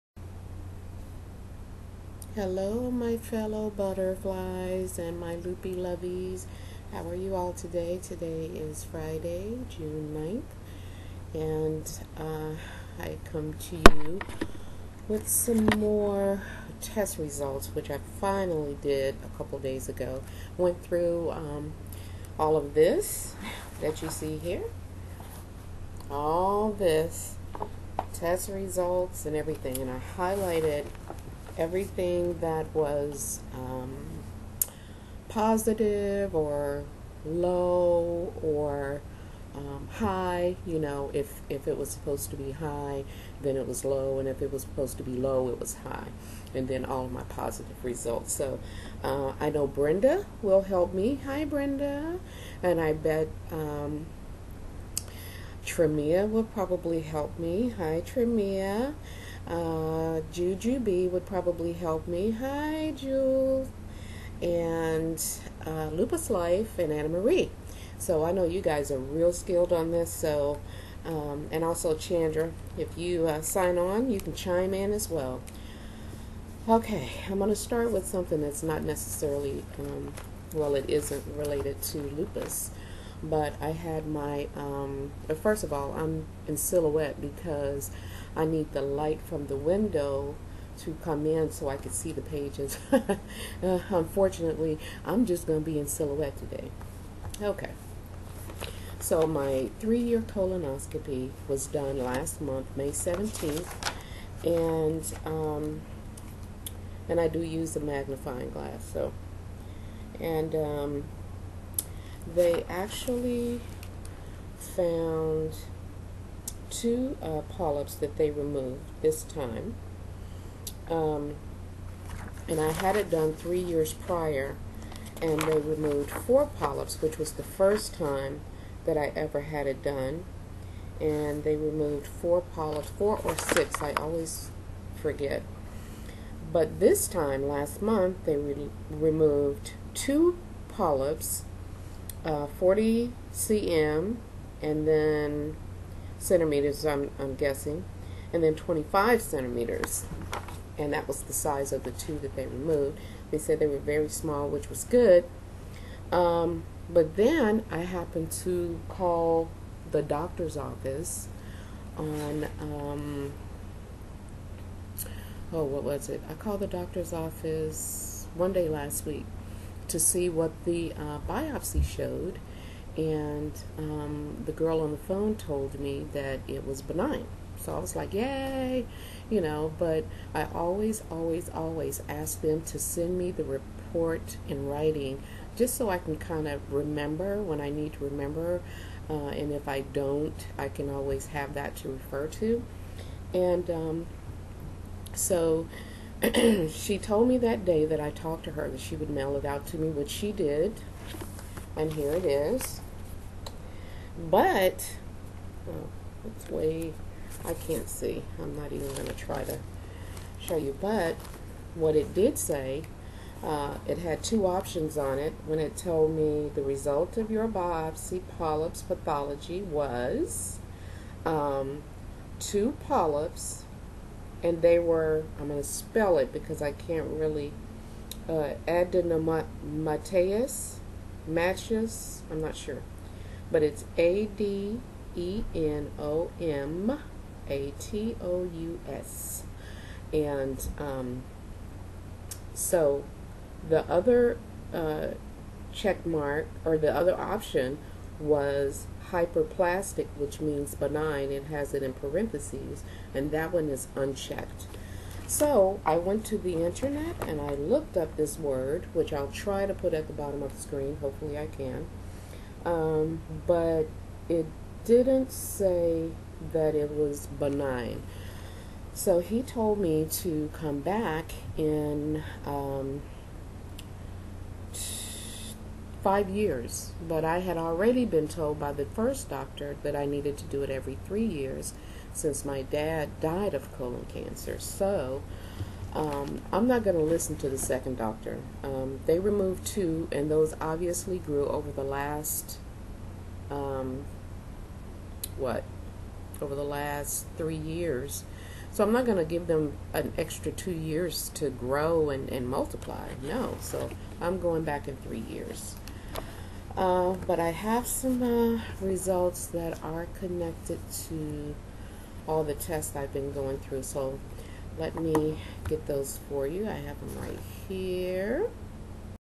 Lupus Journey 6-9-12 Testing, testing, 1, 2, 3!
A whooooole bunch of test results!